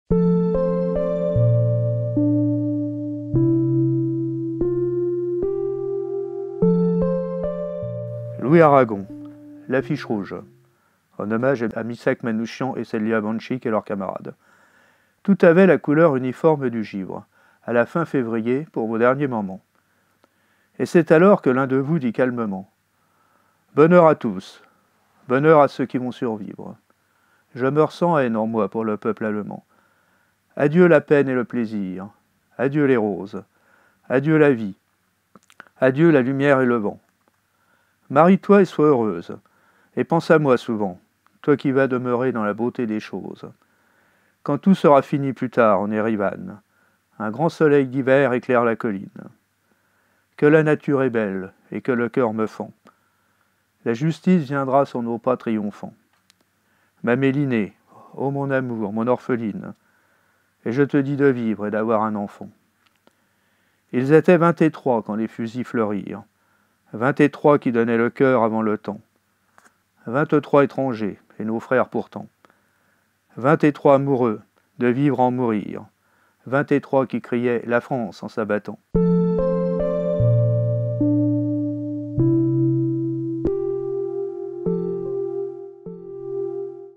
En attendant le Printemps des Poètes... #13 "L'affiche rouge" de Louis Aragon lu par Didier
Du 11 au 27 mars 2023, c'est Le Printemps des Poètes.
Cette manifestation a pour but de "contrer les idées reçues et de rendre manifeste l’extrême vitalité de la Poésie en France".
Comme chaque année, nous participerons !
En attendant, la librairie Maipiu vous offre tout plein de lectures poétiques pour vous démontrer quelles richesses et diversités le mot "poésie" recèle.
Bonne écoute !